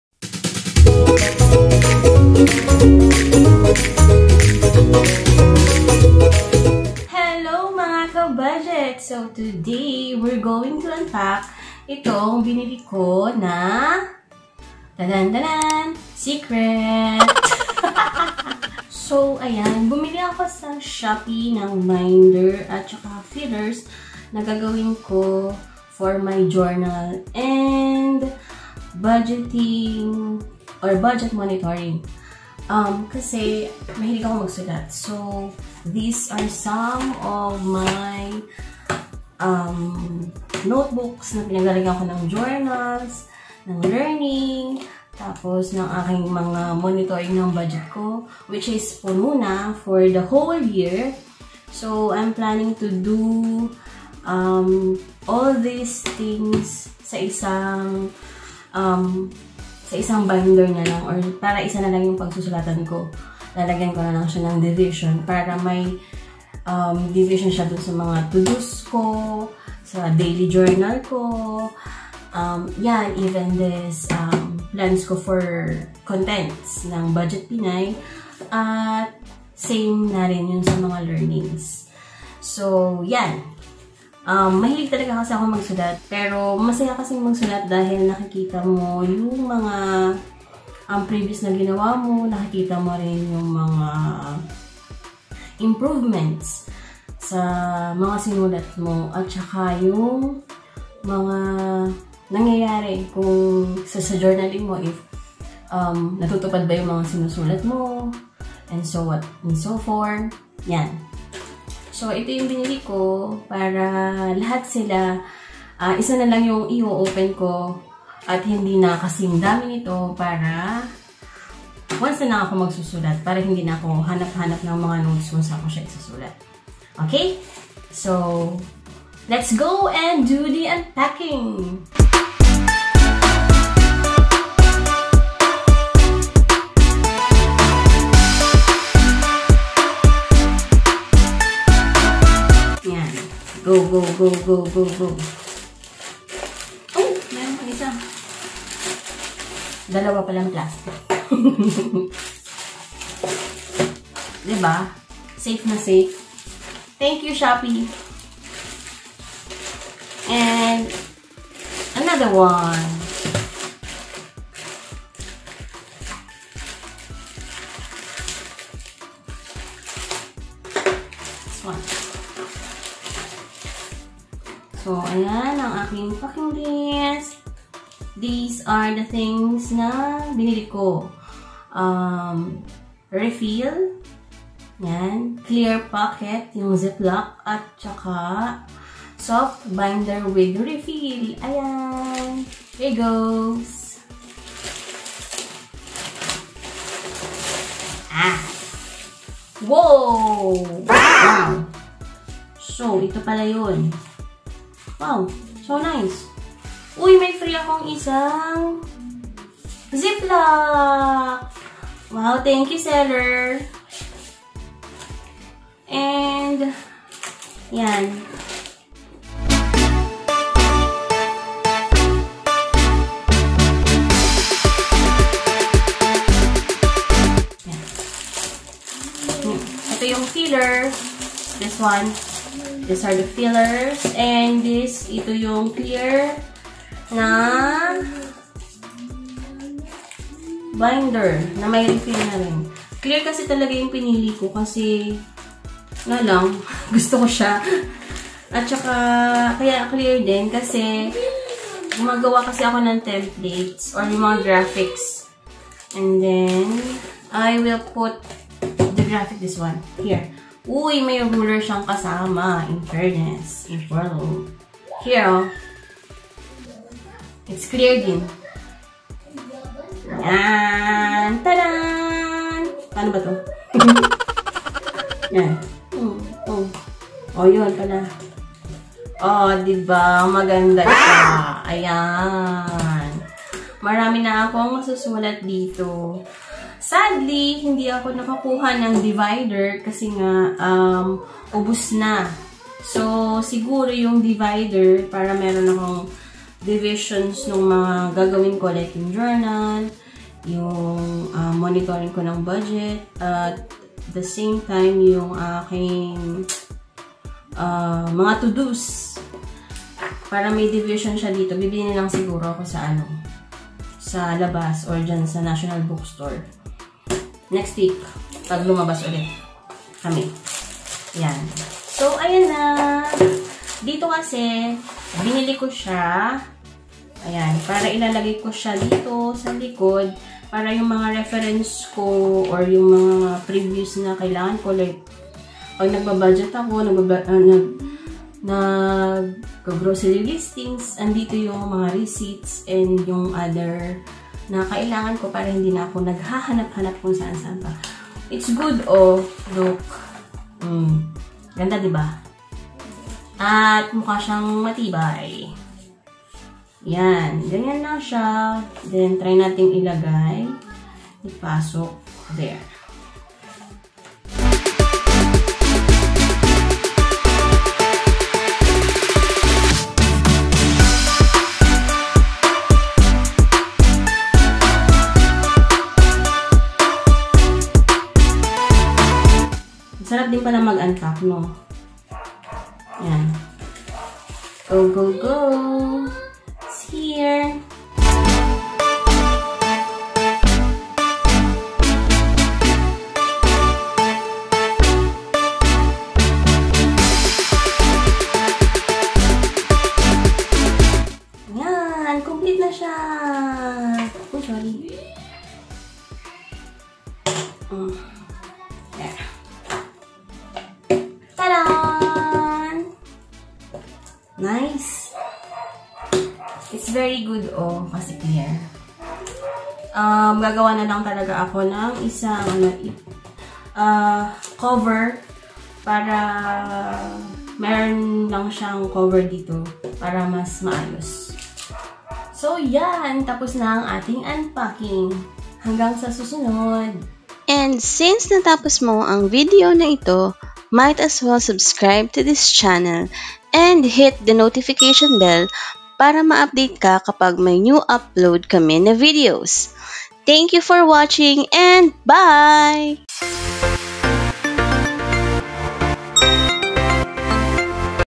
What Makes You Organize | Create your Custom Journal
Let's do the unpacking of my new binder that will be used for organizing my journal, to-do lists, budget monitoring and content plan.